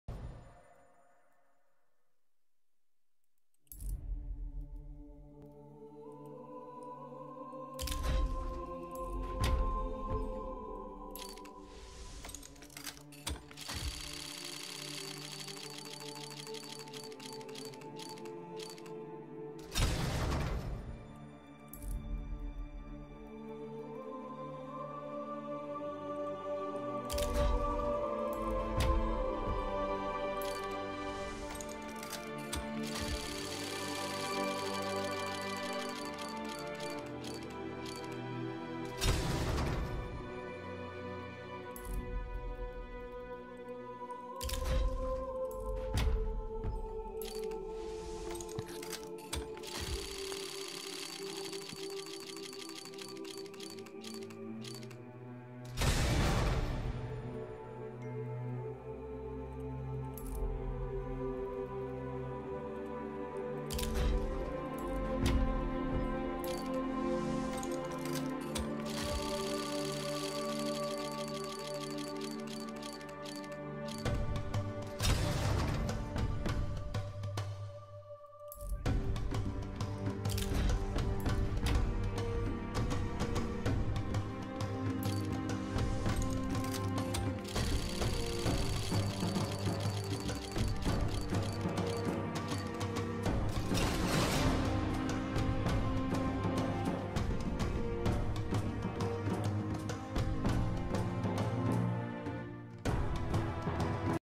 First case opening
decided to open my 7 cases and was quite surprised